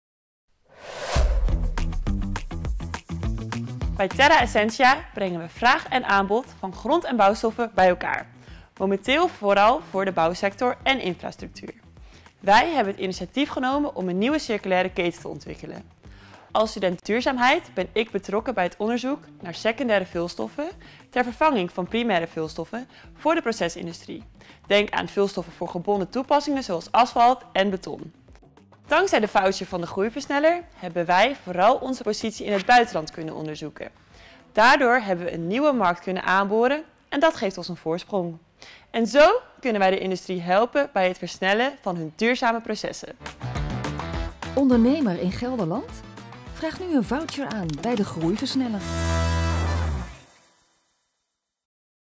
De Groeiversneller: Terra Essentia
Vanuit het hart van Nijmegen brengt Terra Essentia (samen met bijvoorbeeld aannemersbedrijven en producenten van materiaal) de circulaire keten op gang door vraag en aanbod van minerale grond- en bouwstoffen te verduurzamen. Vooral binnen de sectoren bouw en infrastructuur. “De focus ligt op het vervangen van primaire vulstoffen door secundaire. Een van onze drijfveren is dat we voorkomen dat we onder andere onze natuurlijke groeves uitputten en dat waardevol materiaal een tweede leven verdient.”

Dankzij de kennisvoucher van De Groeiversneller heeft Terra Essentia de Radboud Universiteit kunnen inzetten voor onderzoek naar hun marktpositie in vooral het buitenland. Door het betreden van nieuwe markten kan Terra Essentia de industrie in binnen- en buitenland helpen bij het versnellen van duurzame processen. “De start-up van secundaire vulstoffen is met recht een success te noemen.”

Wil je weten wat De Groeiversneller voor jouw bedrijf kan betekenen? Ga naar en dien je groeiplan in.